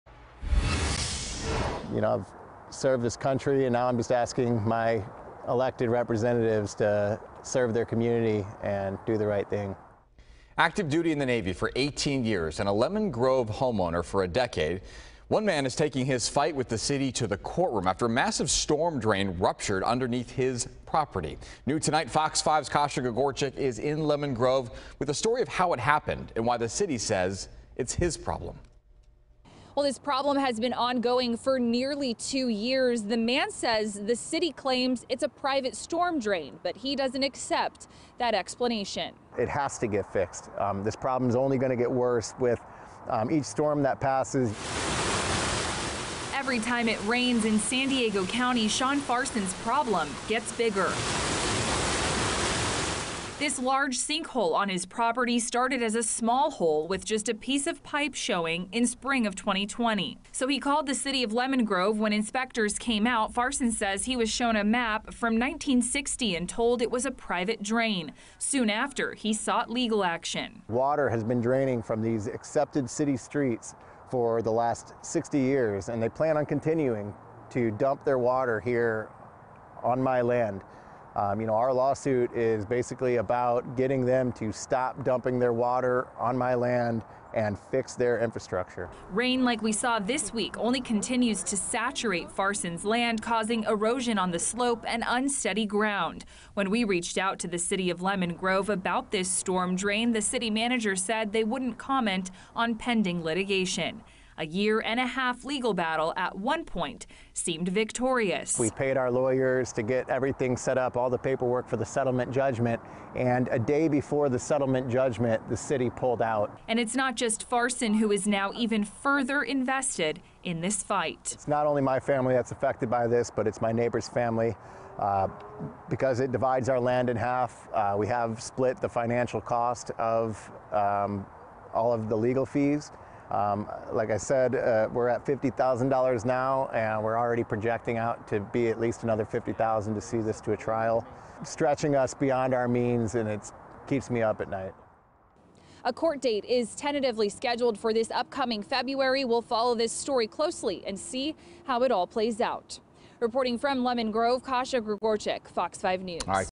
Storm Drain Ruptures On Man's Lemon Grove Property
Kasia Gregorczyk reports on a homeowner's legal battle against the city over the sinkhole in his backyard.